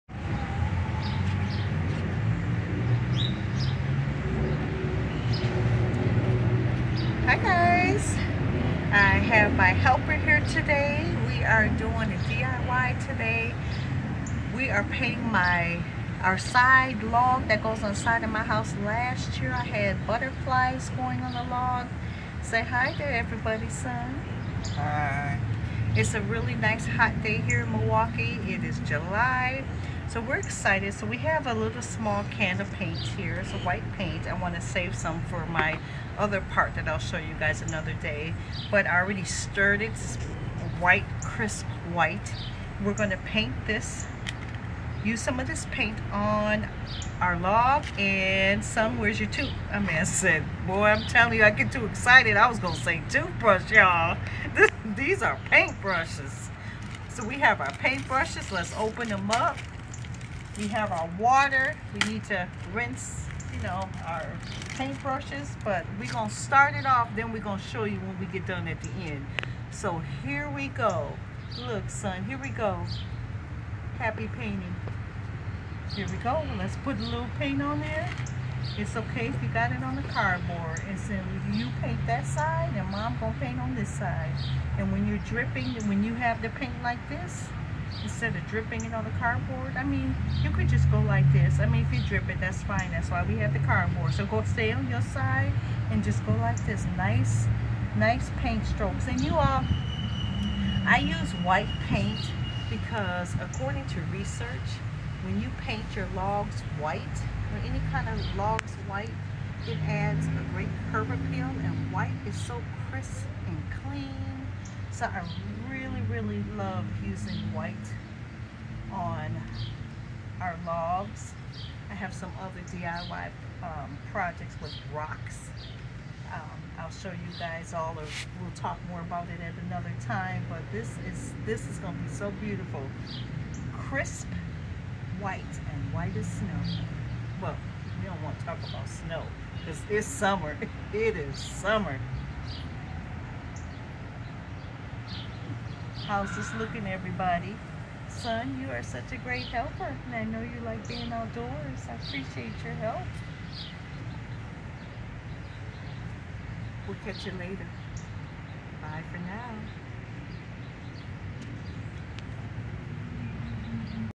Painted our log
Our son did a great job painting the side house log. I use this log for garden decor. Last year, I had a string of butterfly lights on this log.
I was debating this year in whether I should keep it but not getting rid of it any time soon.
Happy DIY friends.